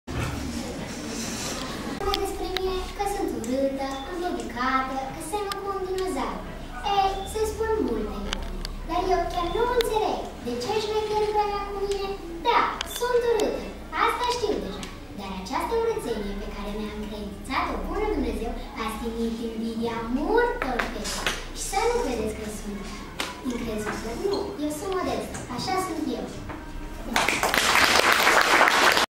Betty cea urata
Galerie foto:
Joi, 8 aprilie 2015, la Școala Gimnazială ,,Grigore Antipa” Botoșani a avut loc activitatea ,,Carnavalul Primăverii”. Elevi din clasele a V-a, a VI-a, a VII-a și a VIII-a au adus la viață personaje celebre, precum Zița, Veta, Spiridon, Zoe, Tipătescu, Cetățeanul turmentat, Albă-ca-Zăpada, Scufița Roșie, Cruela Deville, Zorro și multe altele, prin momente individuale sau de grup, într-un spectacol ce s-a constituit într-un mini-festival de teatru.
Alături de micii actori s-au aflat și profesorii coordonatori, care sunt, totodată, și organizatorii activității : domnișoara Răileanu Oana Elena, doamnele Chiș Silvia și Ursache Liliana, cadre didactice de limba și literatura română. Fundalul sonor a fost asigurat de domnul profesor de educație muzicală Theodor Grădinaru.
Un juriu alcătuit din cadre didactice ale Școlii Gimnaziale ,,Grigore Antipa” a stabilit un clasament, acordând premiile.
Radu Căjvăneanu, coordonatorul Centrului ,,Europe Direct” Botoșani, prezent la Carnaval, în calitate de invitat special, a organizat un concurs-fulger între participanţi cerând să recunoască şi să indice pe harta Europei ţara din care provin autorii textelor interpretate de elevi. Din partea Centrului Europe Direct Botoşani au fost acordate premii în jocuri, ilustrate, hărţi și publicaţii de la Comisia Europeană. Ioana Matfeev, coordonatorul ziarului Buzz, a prezentat publicaţia invitând elevii să se înscrie la cursurile de jurnalism organizate de asociaţie.
Profesor Răileanu Oana Elena,
responsabilul Comisiei Metodice
de Limba Română